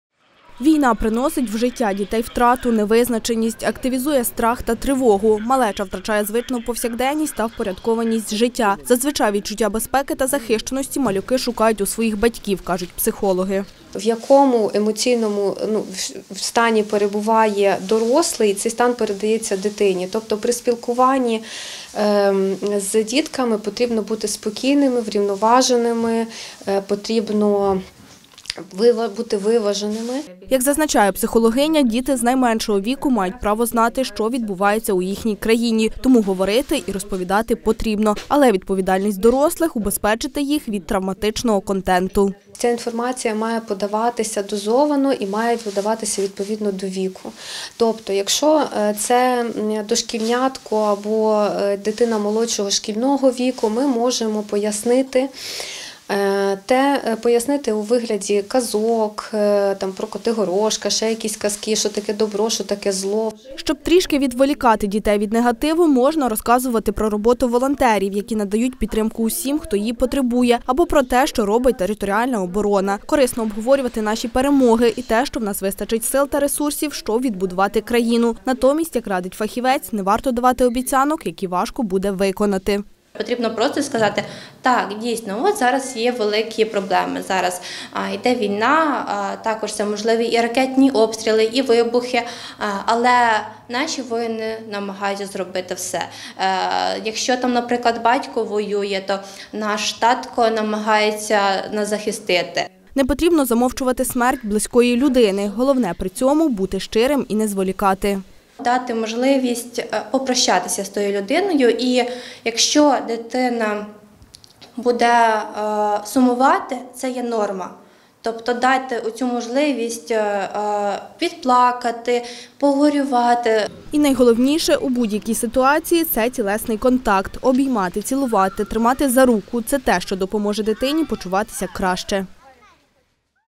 Як говорити з дітьми про війну: рекомендації фахівців
Війна має травматичний характер для всіх громадян, а найбільше – для дітей. Але дорослі можуть мінімізувати її негативний вплив. Адже саме від їх поведінки та реакції залежить спокій малечі. Як говорити з дітьми про війну та як допомогти їм зберегти психічне здоров’я – розповіли у Хмельницькому обласному центрі психологічного здоров’я.